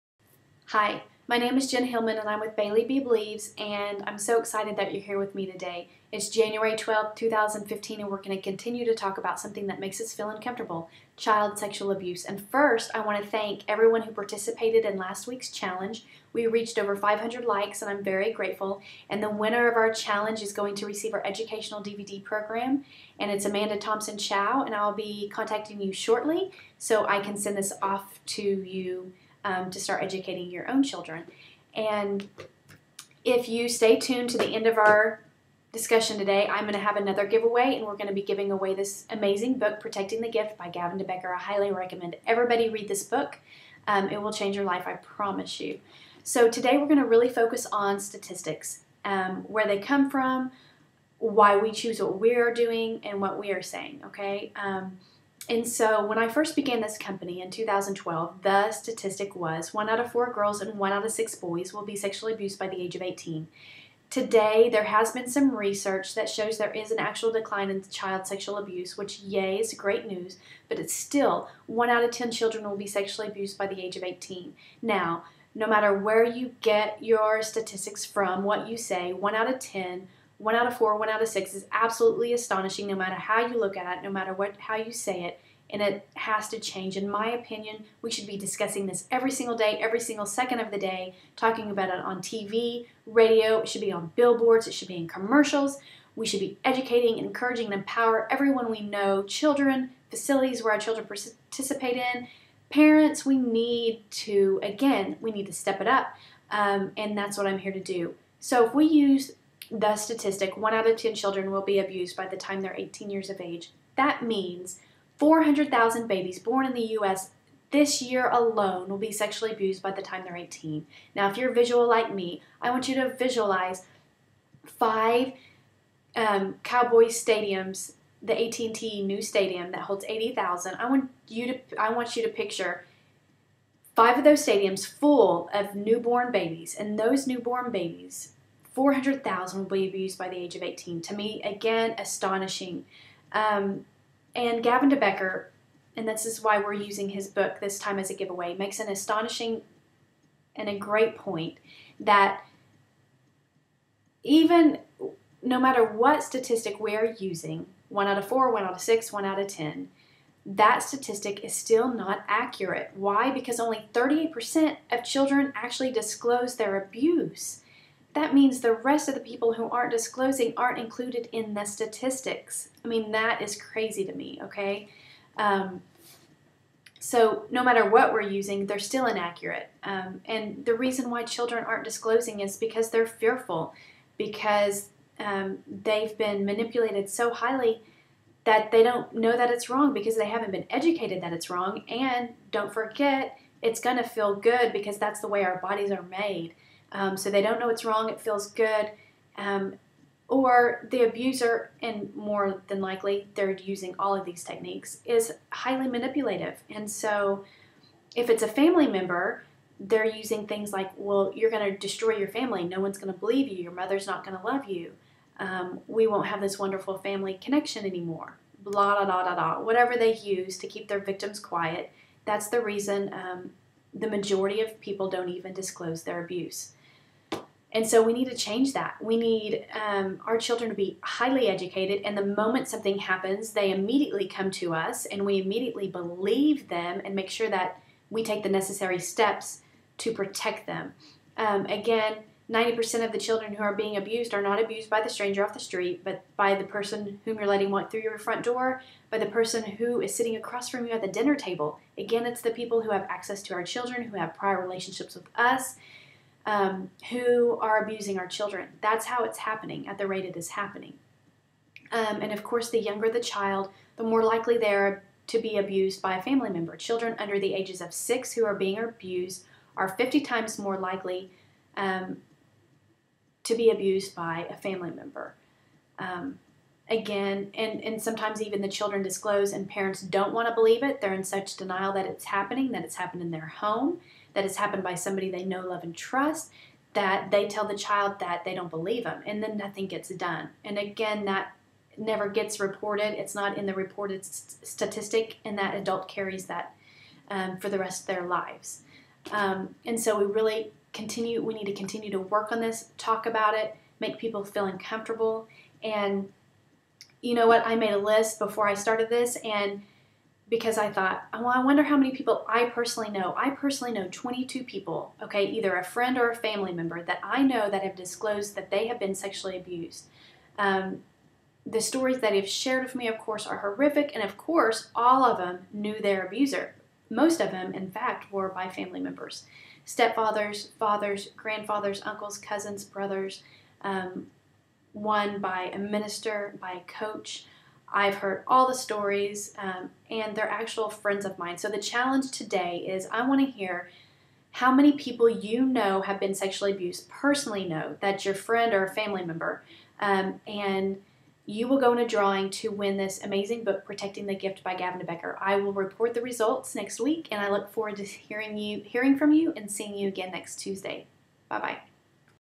What Are the Statistics of Child Sexual Abuse?
Jennifer Hillman, with Bailey Bee Believes, discusses the old and new statistics of child sexual abuse and what that means for society. You can also follow her on FB and Twitter: